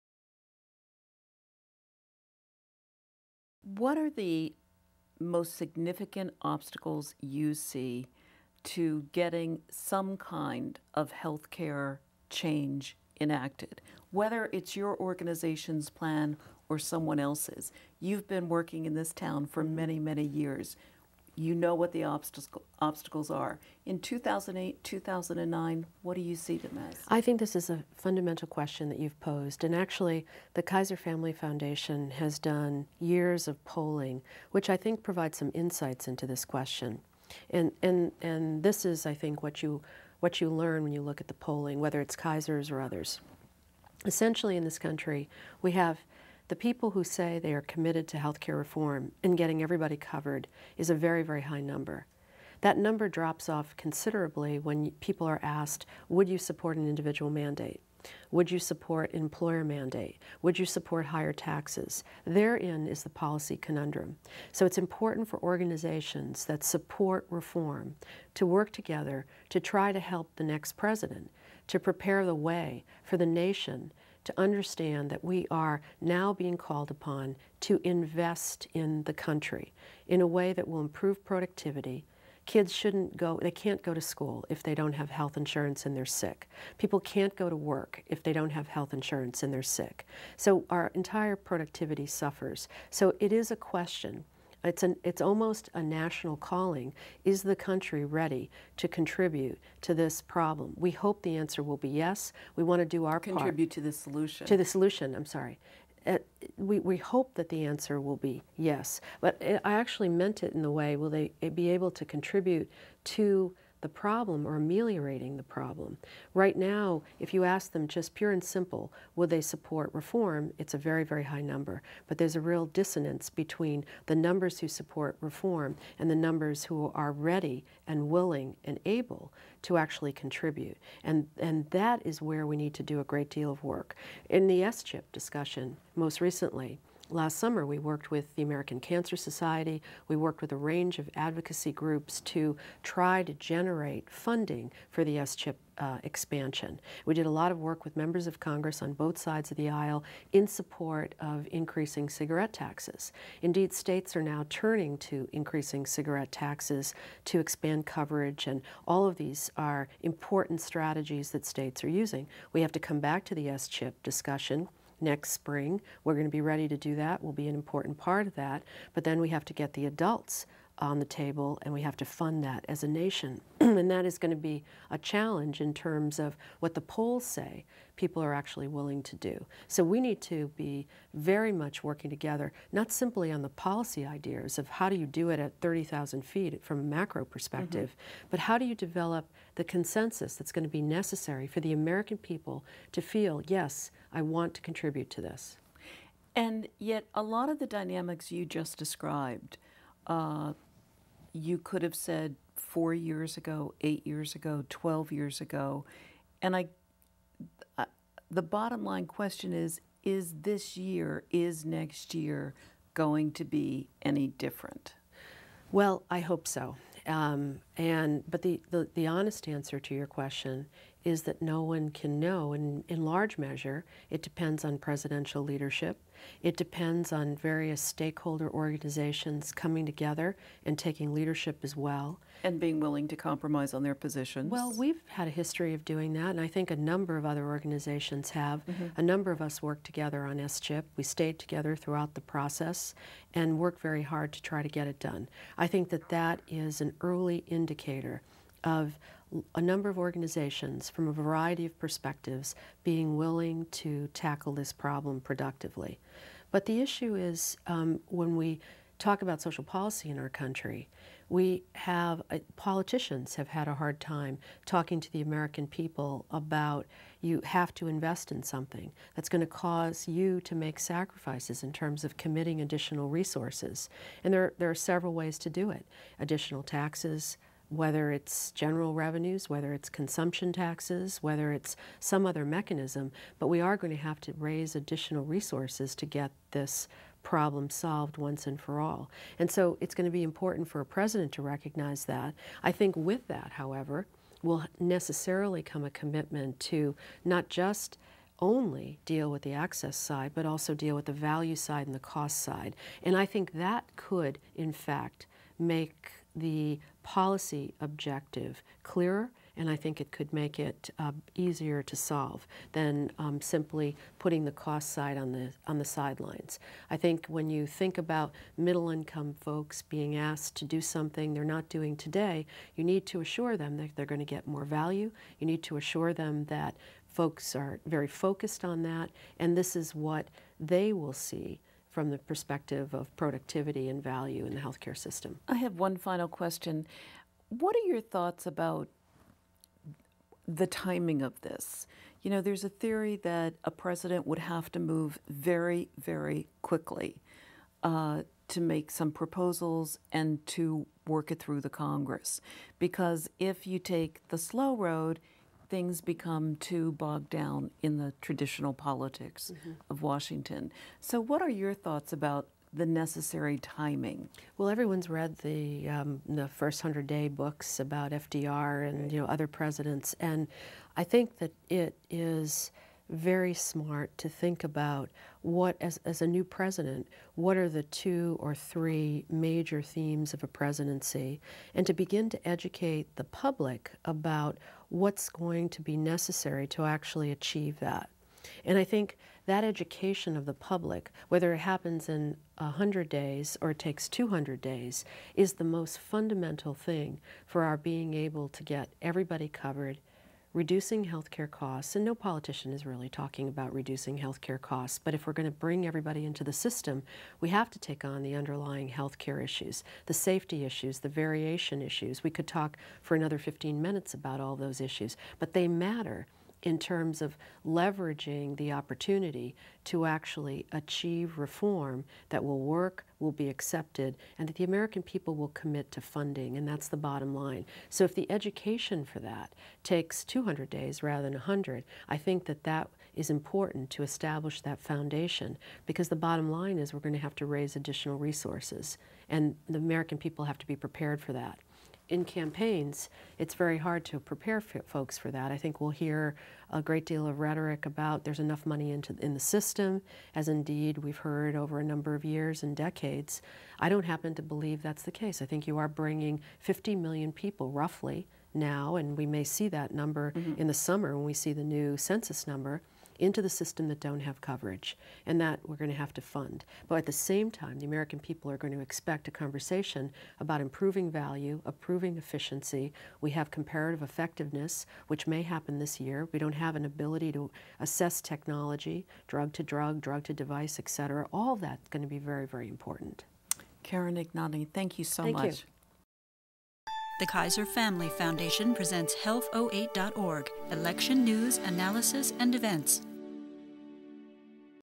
AHIP's Karen Ignagni on the Health Care Debate (Part 2)
Karen Ignagni, president of America's Health Insurance Plans (AHIP), a trade association representing insurers, talks with the Foundation's Jackie Judd about
AHIP's plan for broader coverage, the importance of organizations coming together to achieving change and what government and the next President need to do to change the status quo.